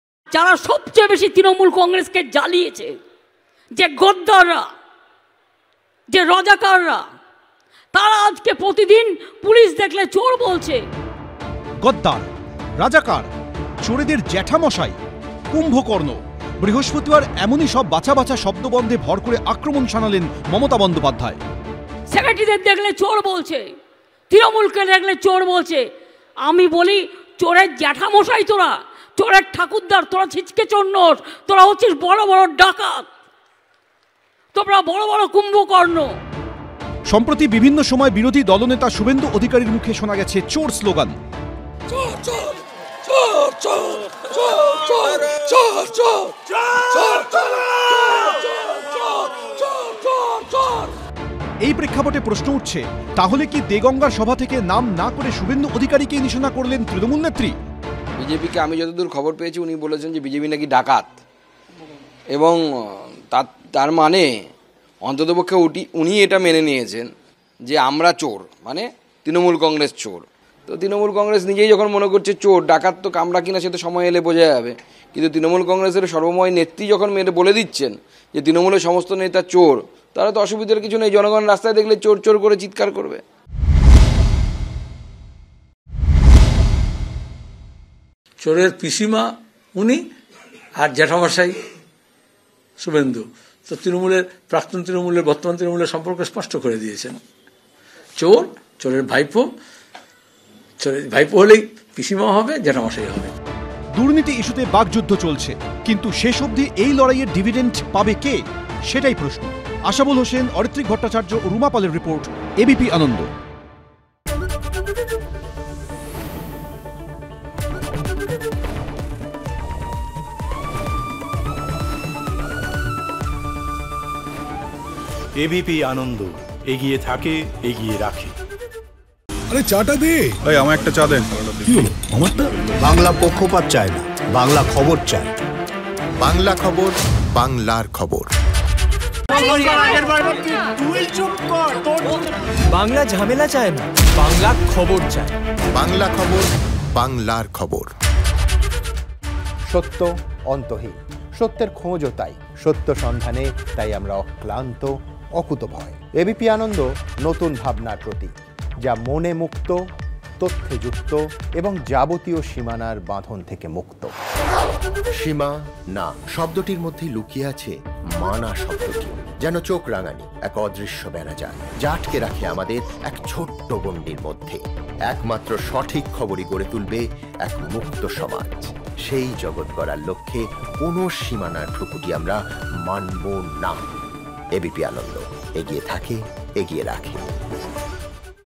TMC: যাঁরা সবচেয়ে বেশি তৃণমূলকে জ্বালিয়েছে, তাঁরা প্রতিদিন পুলিশকে চোর বলছে: মমতা বন্দ্যোপাধ্যায়
যারা সবচেয়ে বেশি তৃণমূল (TMC) কংগ্রেসকে জ্বালিয়েছে, যে গদ্দাররা, যে রাজাকাররা, তাঁরা প্রতিদিন পুলিশকে চোর বলছে। দেগঙ্গায় তৃণমূলের কর্মিসভায় এই সমস্ত শব্দবন্ধে আক্রমণ শানালেন মমতা বন্দ্যোপাধ্যায়। তৃণমূল থেকে বেরলেই গদ্দার হয়ে যায়। পাল্টা মন্তব্য করলেন শমীক ভট্টাচার্য।